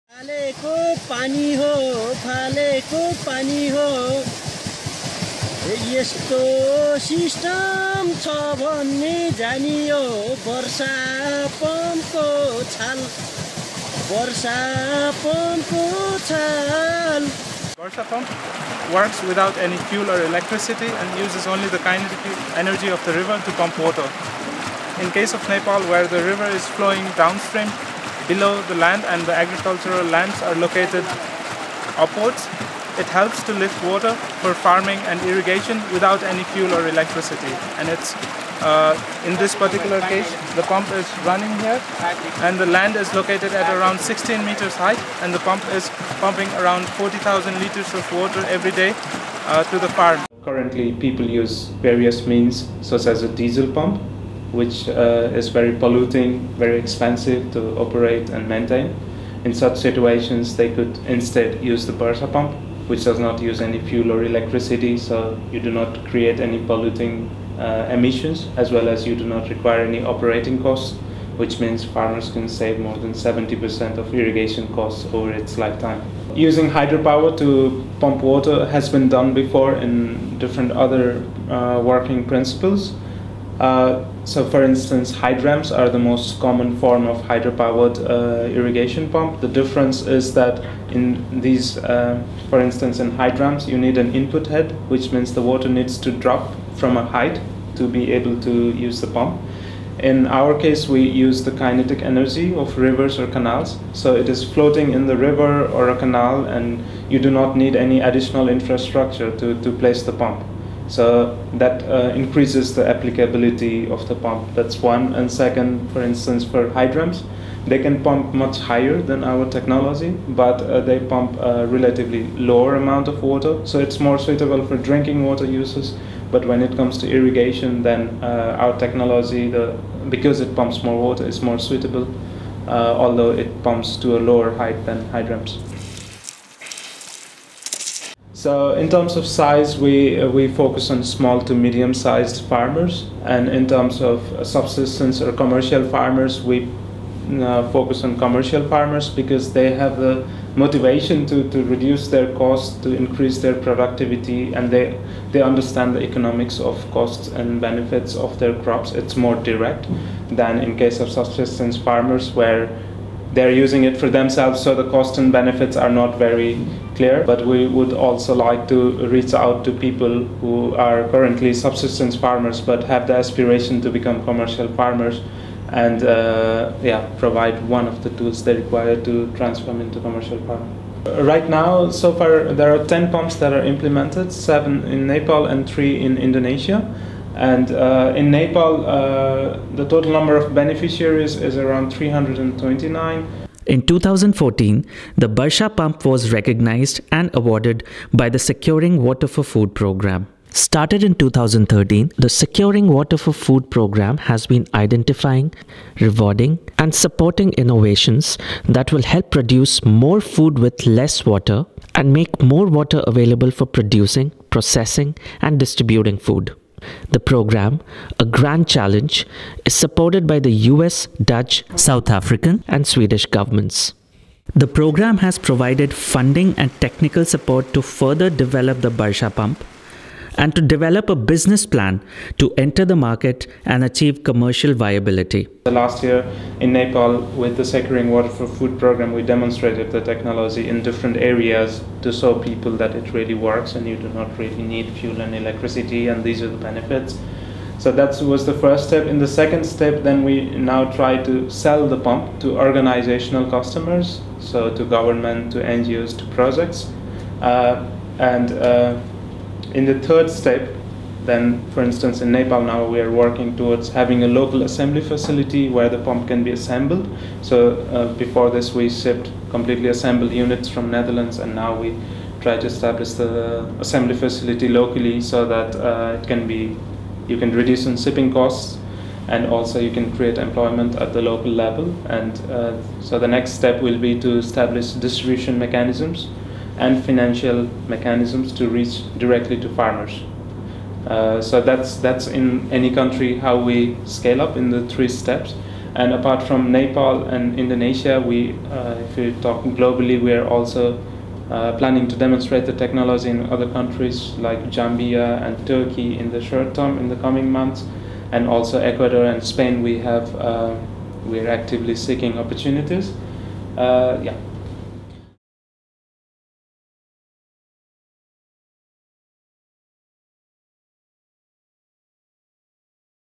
The Barsha Pump
The ‘Barsha’ Pump pumps water from a flowing river, stream, or canal to agricultural fields at a higher elevation. It does not use fuel or electricity; it runs using the kinetic energy of the flowing water.

The pump has been designed and developed by aQysta, a Netherlands-based company. It has been introduced in the Nepal market with support from the Securing Water for Food programme.

Produced by: TheWaterChannel
Year: 2015
Language: English, Nepali
Region: Nepal